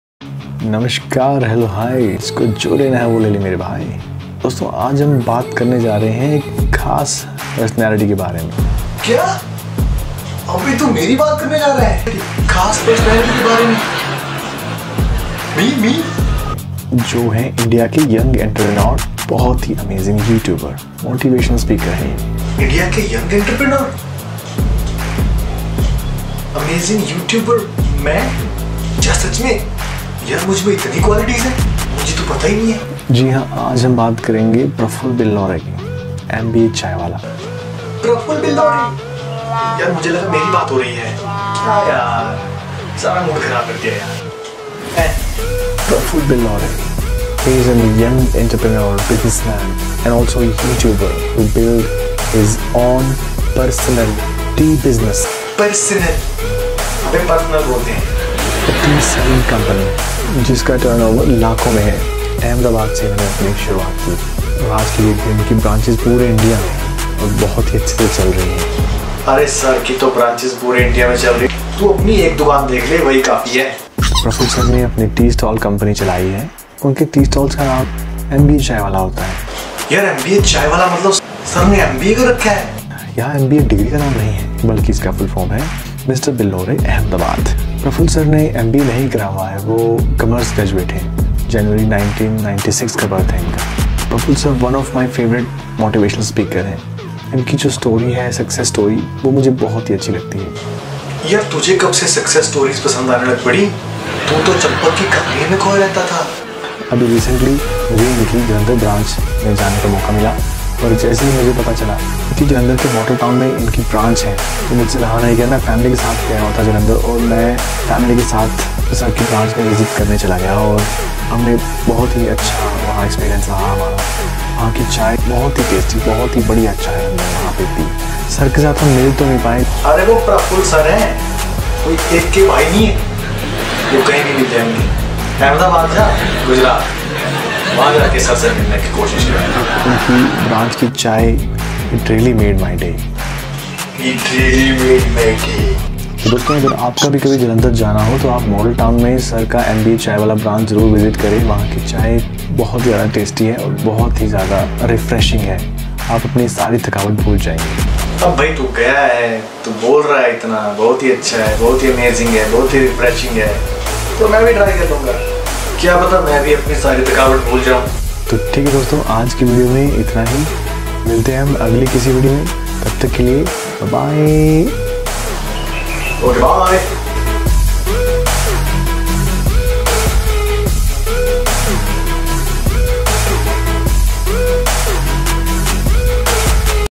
Tea at MBA Chai Wala II Best experience at Model Town Jalandhar
In this video I am telling in brief about an Indian business entrepreneur, a you tuber and a motivational speaker Mr. Praful Billore. Mr. Praful Billore is also known as MBA Chai Wala. Today MBA Chai Wala is a huge brand whose tea business is spread all over India and going to be spread all over world in coming years of future. I am personally influenced by motivational speeches of MBA Chai wala (Mr. Praful Billore) and his success story. That is why I came to make this video.
Thanks for visiting Learning To Success with Fun youtube channel.